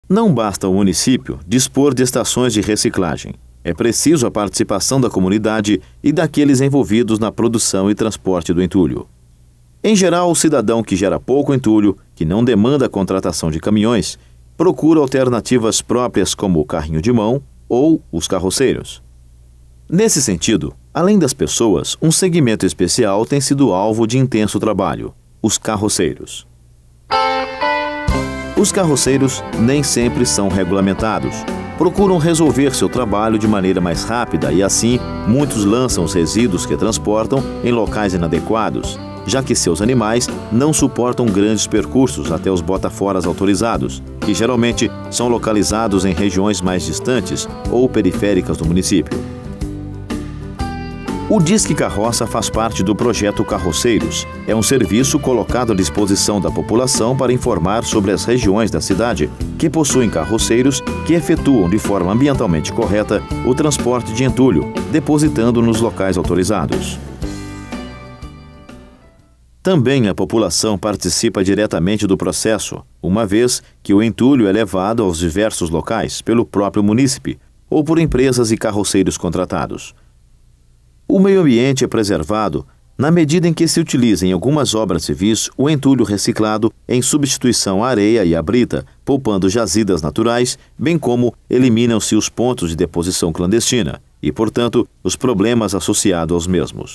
Mobilidade Social da Reciclagem - Curso a Distância Reciclagem de Entulho CPT
Mobilidade social da reciclagem - A atuação da população no sentido de levar os resíduos da construção civil aos locais adequados demanda um investimento permanente na divulgação dos endereços das Unidades de Recebimento e das Estações de Reciclagem. Nesse sentido, um segmento especial tem sido alvo de intenso trabalho por parte da SLU - os carroceiros.